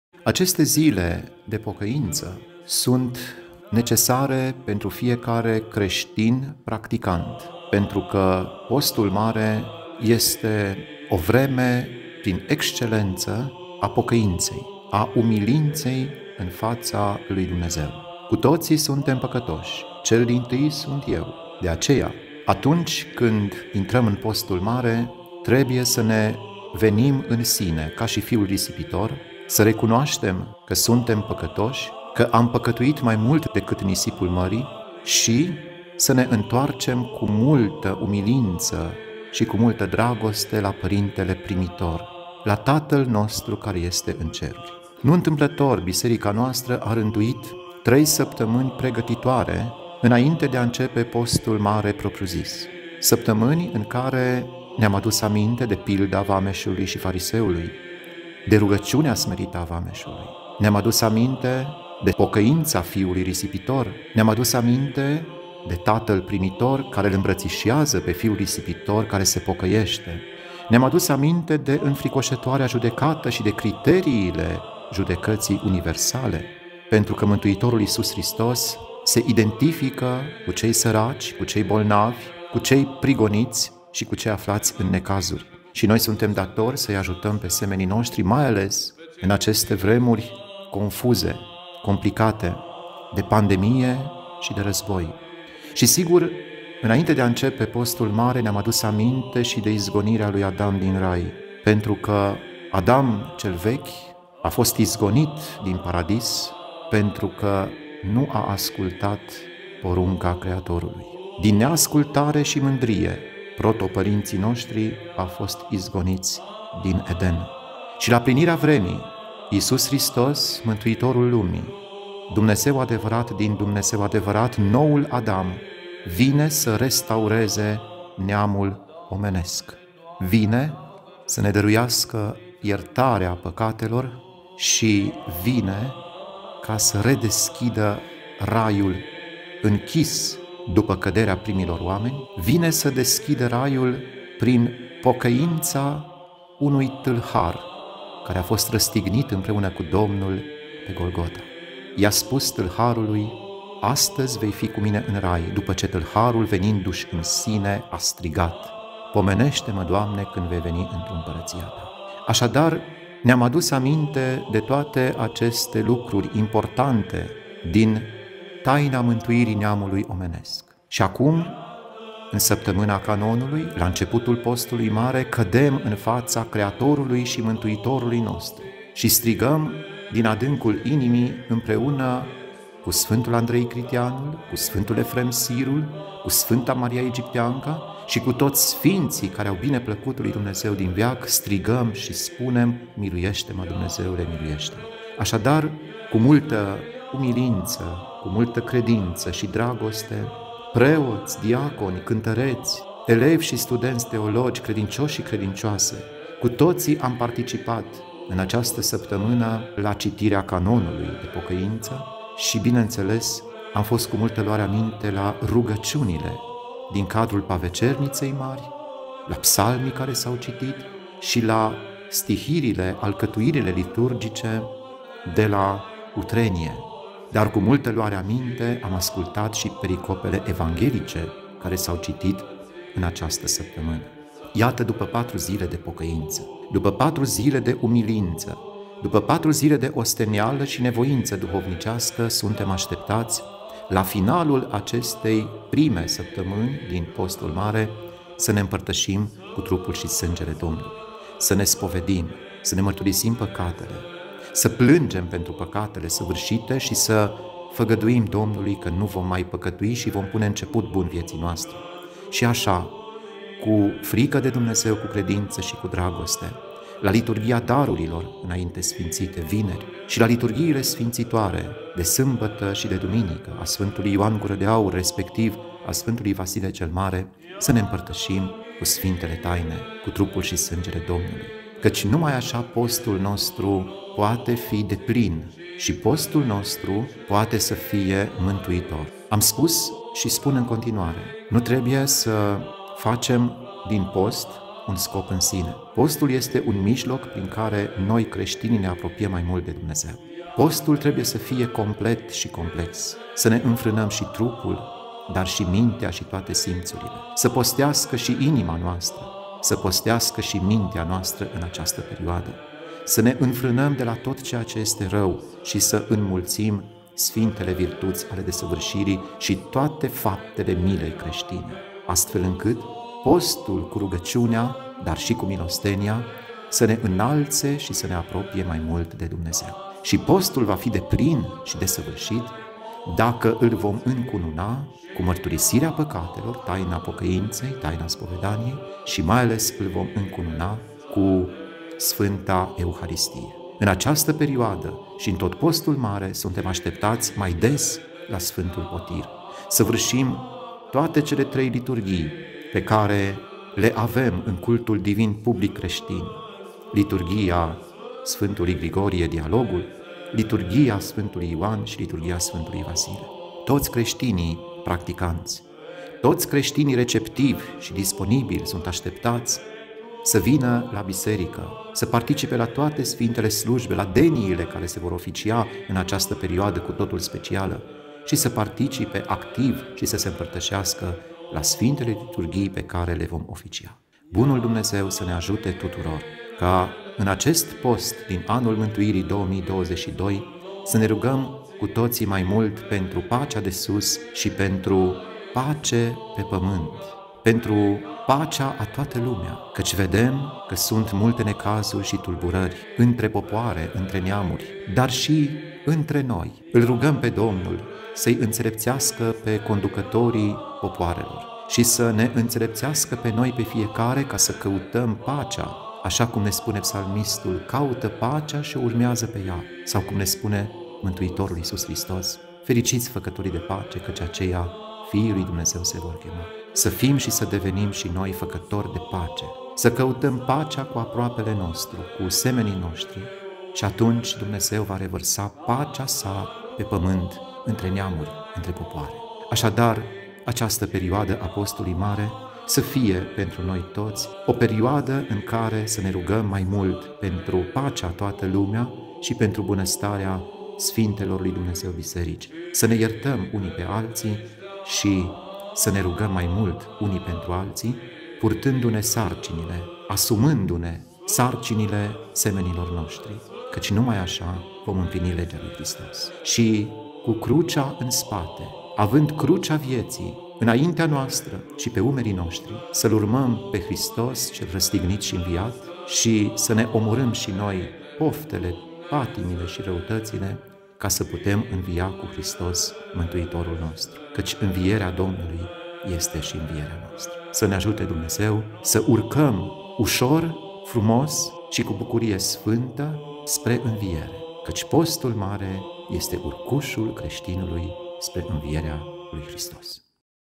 Cuvant duhovnicesc in prima saptamana a Postului Mare
Cuvânt duhovnicesc al Preasfințitului Părinte Lucian, Episcopul Caransebeșului în prima săptămână a Postului Mare